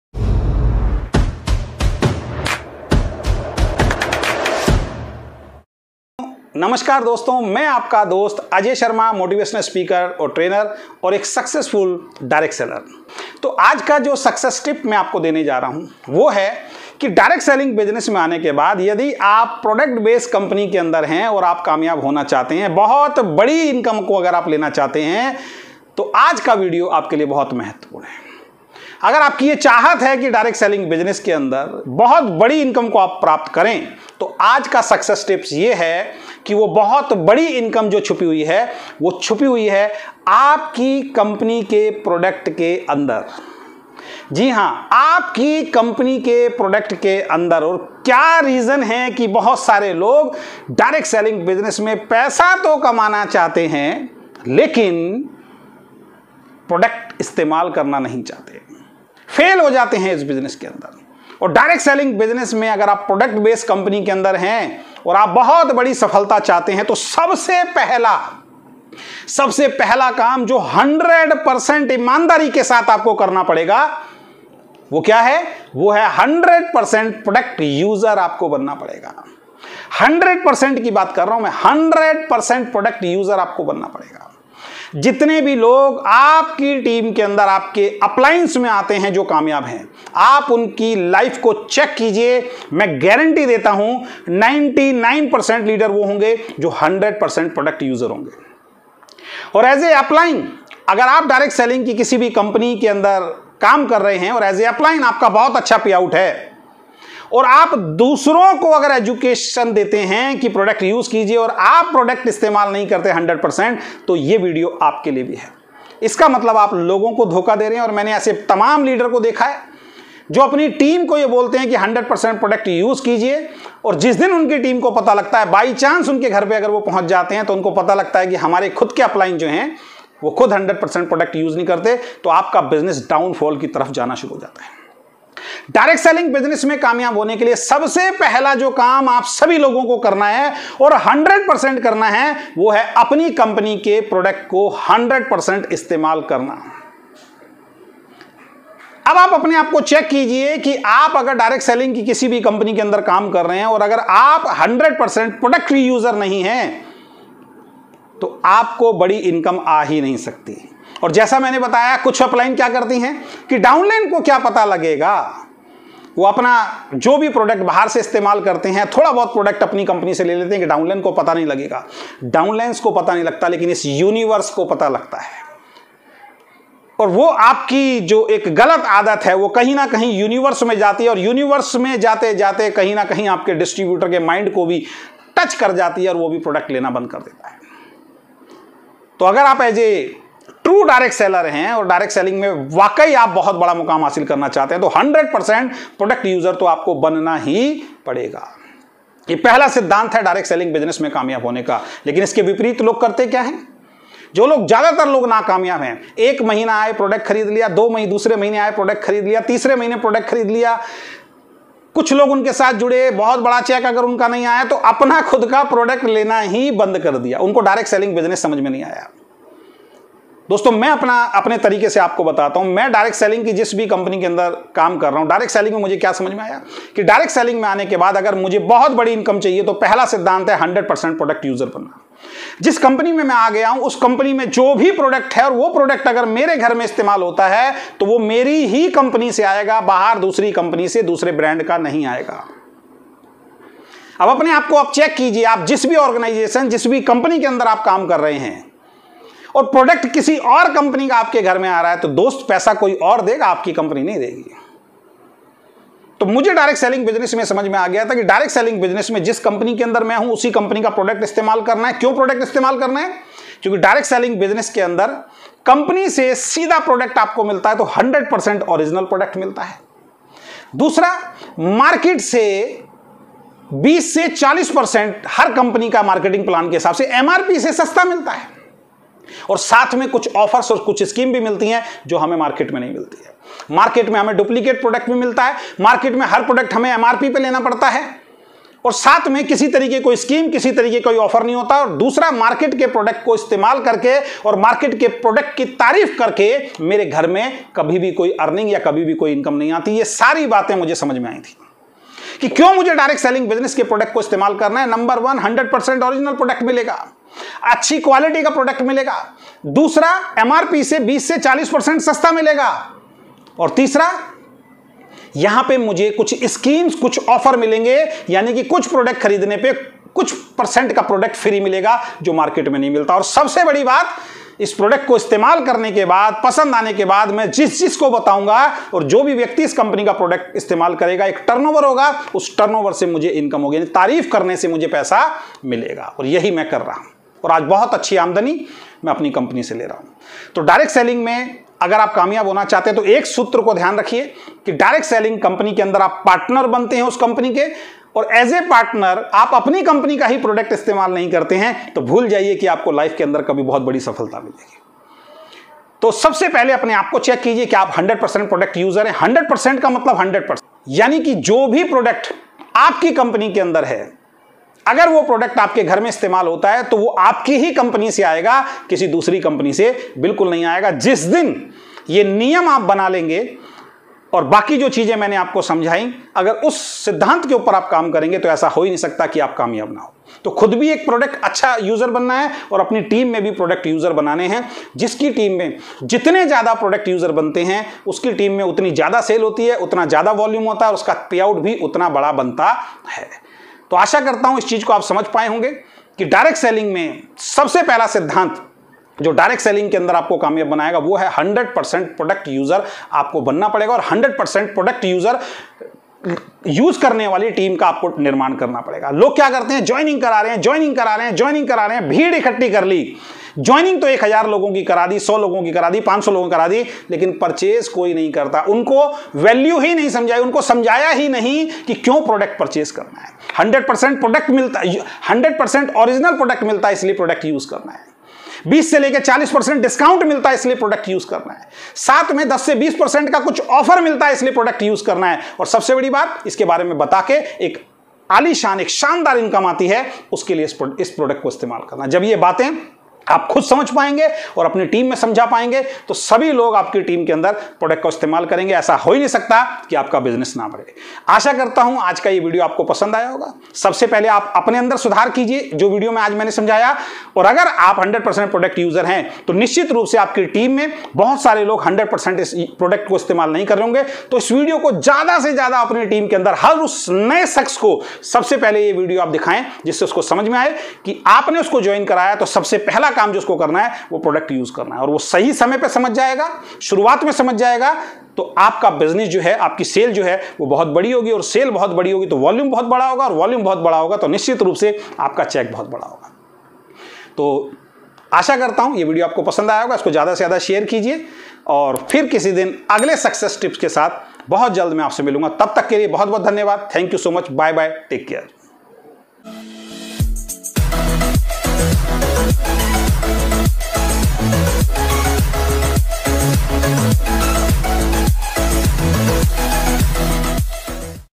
प्रोडक्ट इस्तेमाल करना क्यों जरूरी है ? || Network Marketing || Ajay Sharma
Hello My Dear friends aj ham baat karenge kyu hame networking business products kyu use karna chahiye , dosto agar apko a video pasand ata hai toh channel ko subscribe jaruru kijiye aur sath mai like comment and sharna mat bhulna